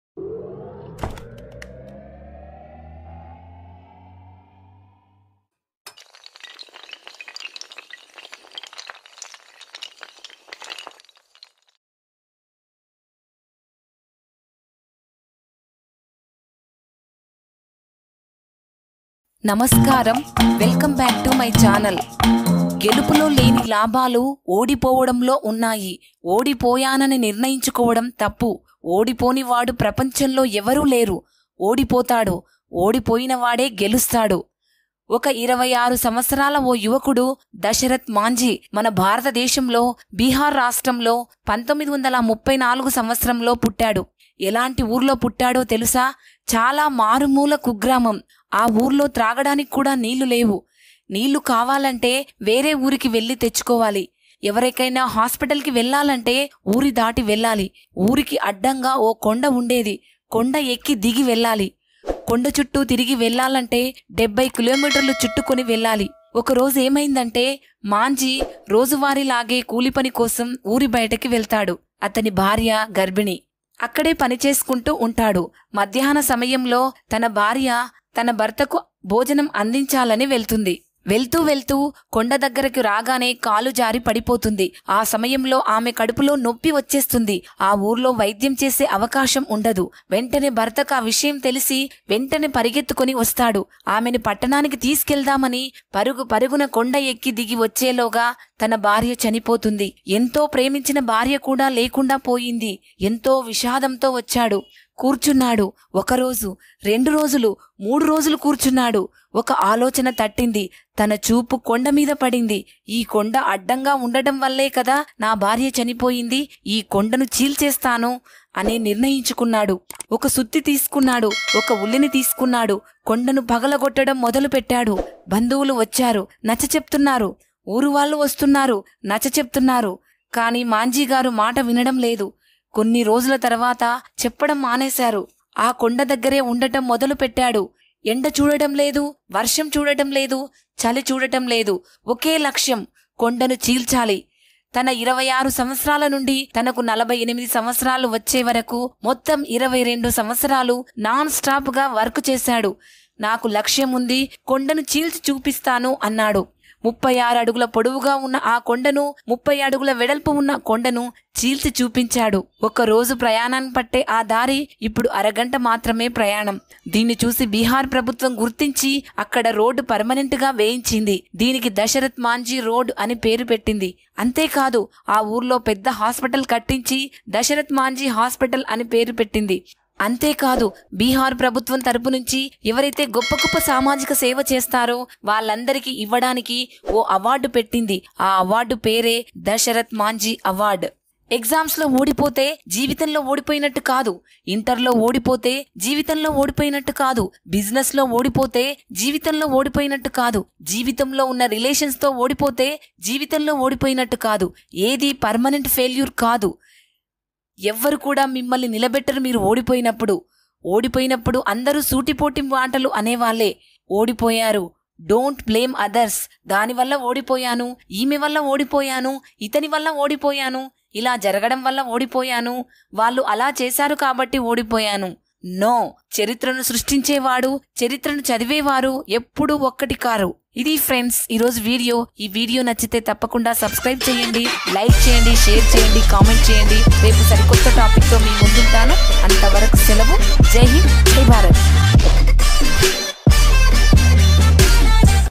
Failure || Mountain Man || Success
Hi Motive Listeners
Today I Hope this video Helps

Topics Covered :-
failure
success
don't blame others
history
dasaratath mahnji
mountain man